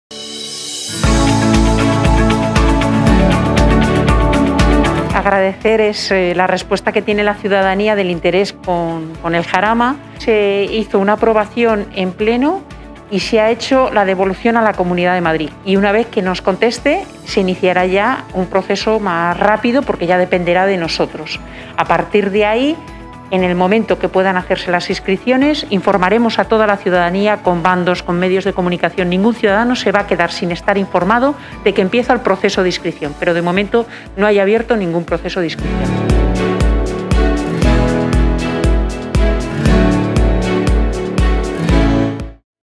Información sobre las inscripciones para solicitar vivienda en el Barrio del Jarama
El Pleno extraordinario de  este mes de enero  el Ayuntamiento de Coslada aprobó la resolución de las alegaciones definitivas del Plan General de Ordenación Urbana relativas al futuro Barrio del Jarama.
Pendientes de su devolución una vez revisado por la Comunidad de Madrid, la presidenta de EMVICOSA pide paciencia a los vecinos y promete comunicar en tiempo y forma para que quien quiera apuntarse, pueda hacerlo.